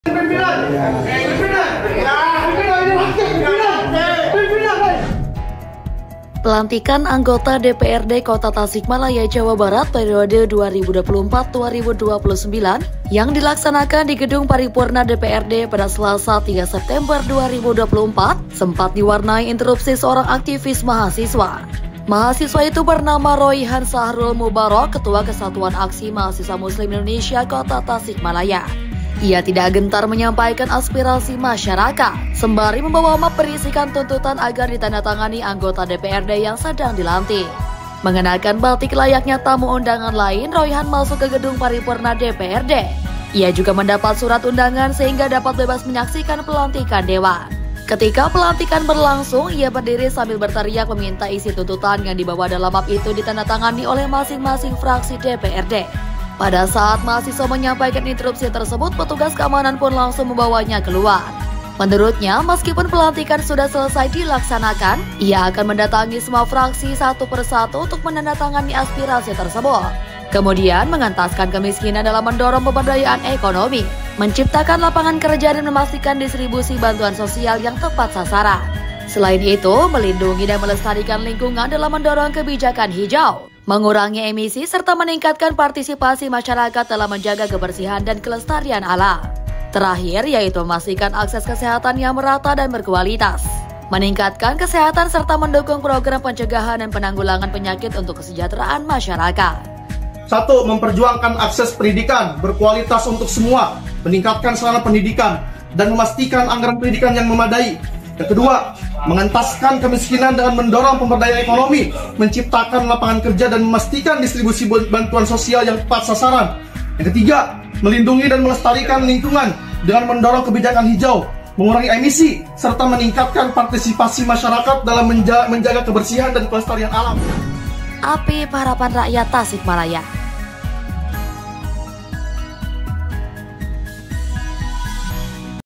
Interupsi Mahasiswa Warnai Pelantikan Anggota DPRD Kota Tasikmalaya
Pelantikan anggota DPRD Kota Tasikmalaya, Jawa Barat, periode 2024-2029 yang dilaksanakan di Gedung Paripurna DPRD, Selasa (3/9/2024) sempat diwarnai interupsi seorang aktivis mahasiswa.

Mahasiswa itu bernama Royhan Sahrul Mubarok Ketua Kesatuan Aksi Mahasiswa Muslim Indonesia (KAMMI) Kota Tasikmalaya. Ia tidak gentar menyampaikan aspirasi masyarakat, sembari membawa map berisikan tuntutan agar ditandatangi anggota DPRD yang sedang dilantik.

Mengenakan batik layaknya tamu undangan lain, Royhan masuk ke gedung paripurna DPRD. Ia juga mendapat surat undangan sehingga dapat bebas menyaksikan pelantikan dewan. Ketika pelantikan berlangsung, ia berdiri sambil berteriak meminta isi tuntutan yang dibawa dalam map itu ditandatangani oleh masing-masing fraksi DPRD.